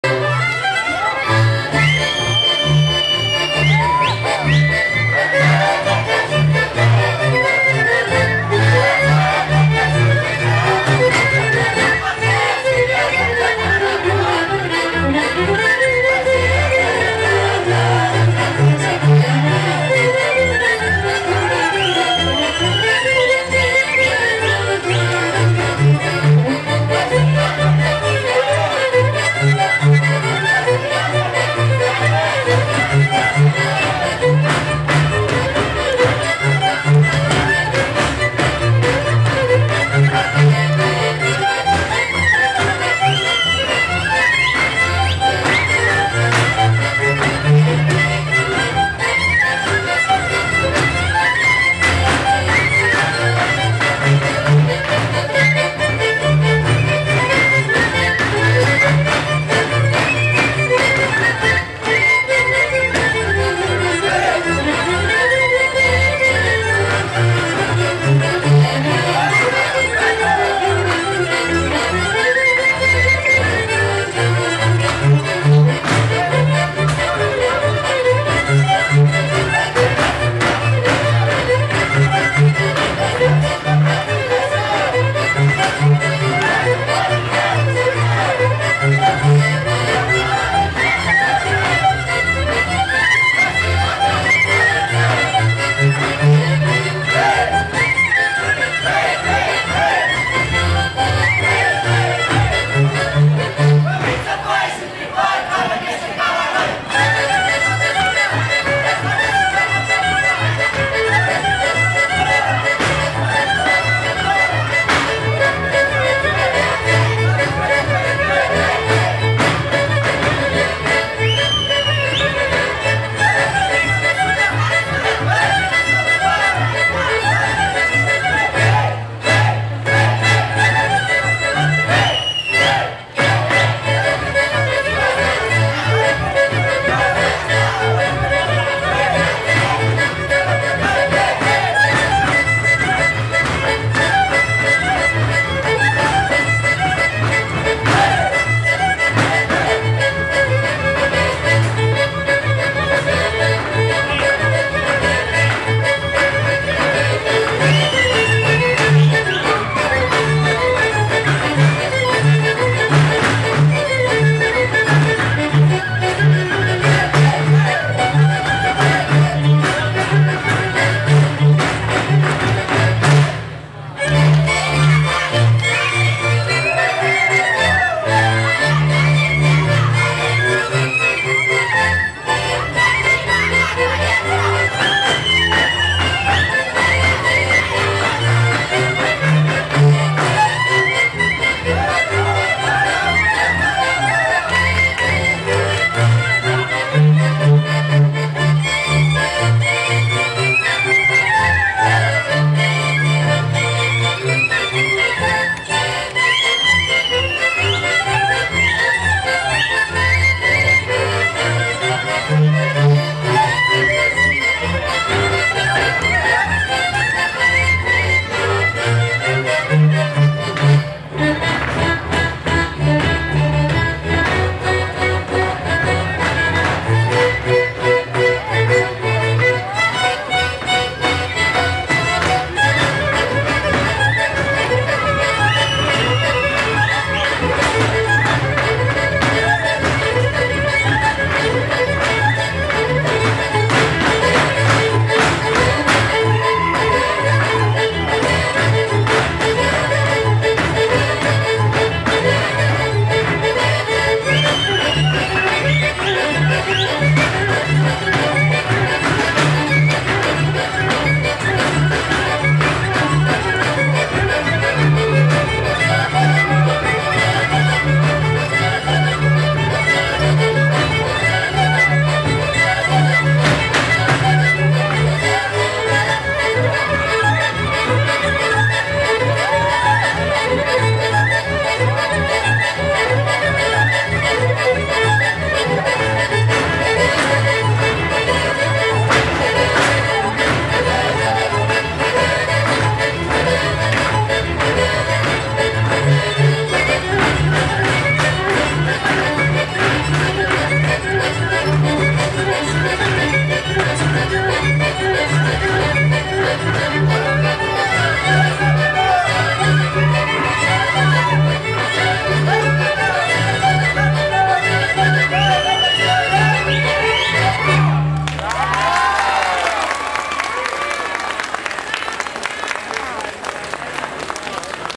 Grupul Romanasul (III)- Padova 2009
Grupul Romanasul, alaturi de Fundatia Culturala Dana Ardelea si Ansamblul folcloric Doina, la Padova-Italia 2009, partea III